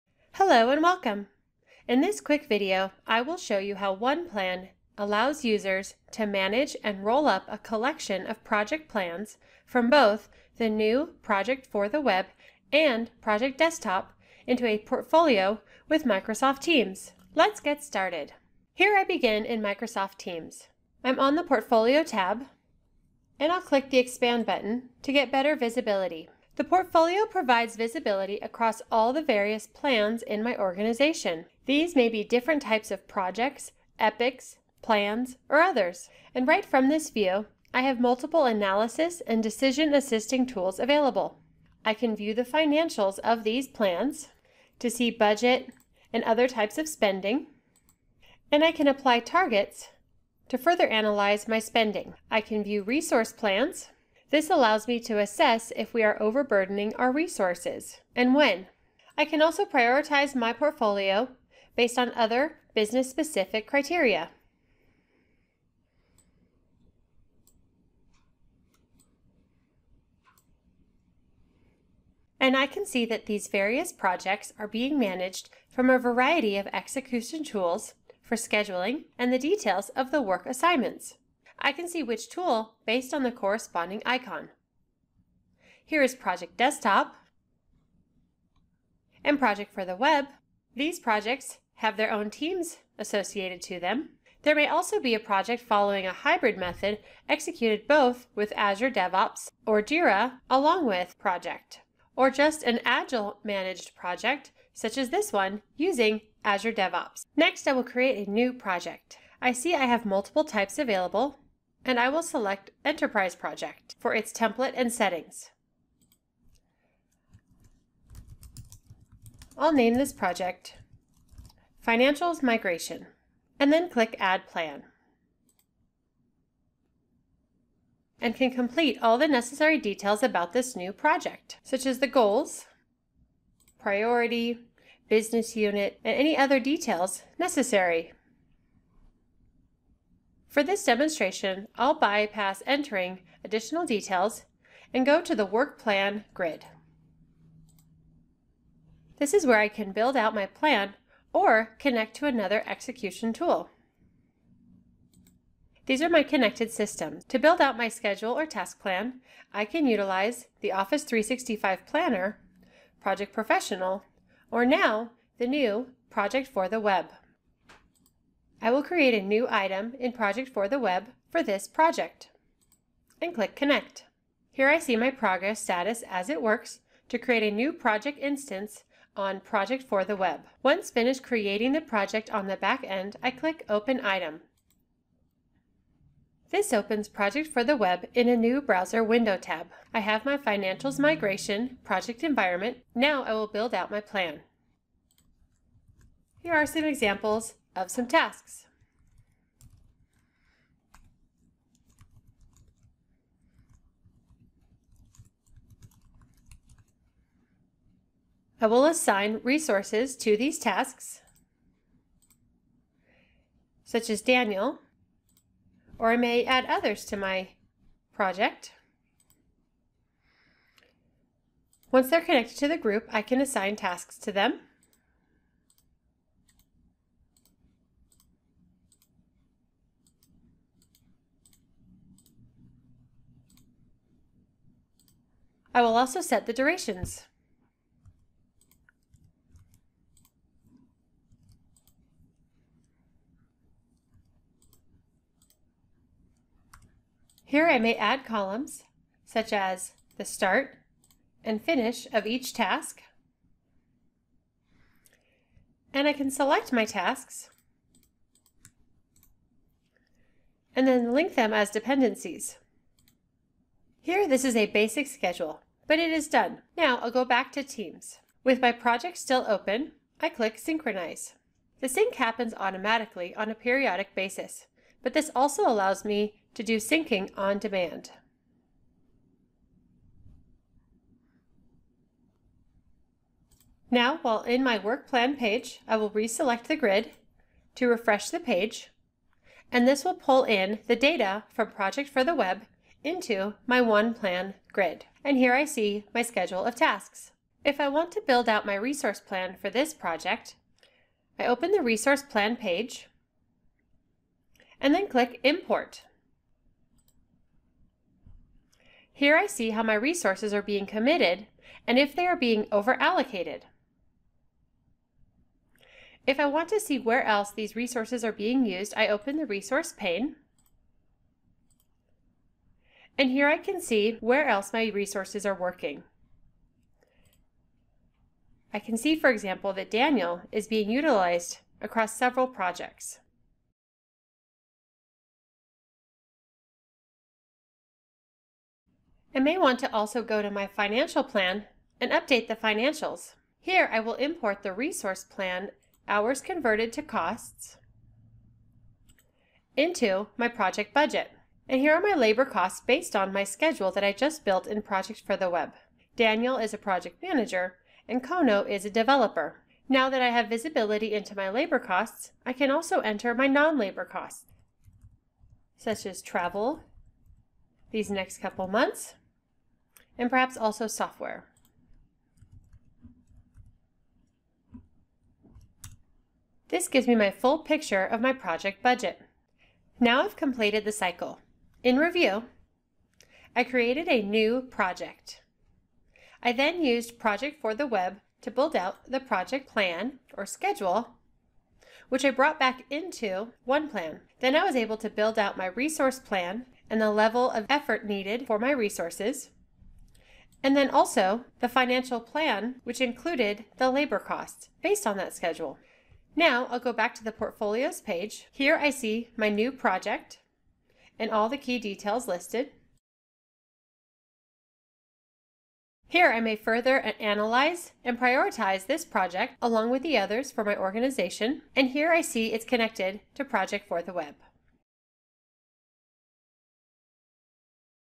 Microsoft Project for the Web with Microsoft Teams and OnePlan
In this quick video, I will show you how OnePlan allows users to manage & roll up a collection of project plans from both the new Project for the Web and Project Desktop into a portfolio with Microsoft Teams.